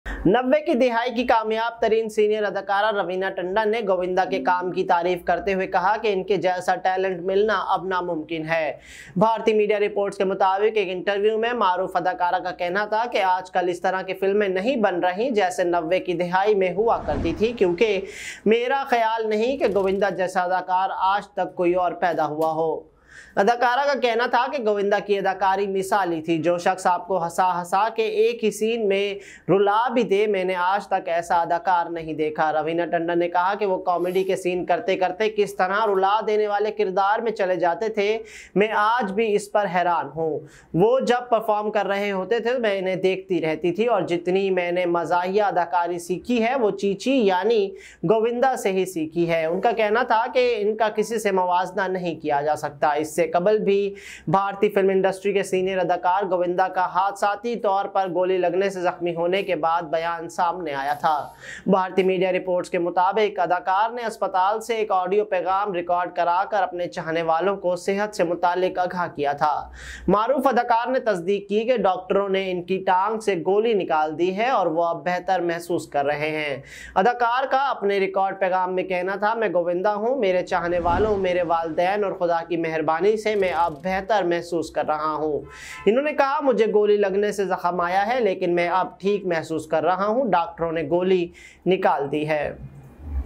Raveena said Bollywood has stopped making movies that can use Govinda talent | Wahjoc Entertainment
Raveena Tandon said Bollywood has stopped making movies that can use Govinda's talent | Wahjoc Entertainment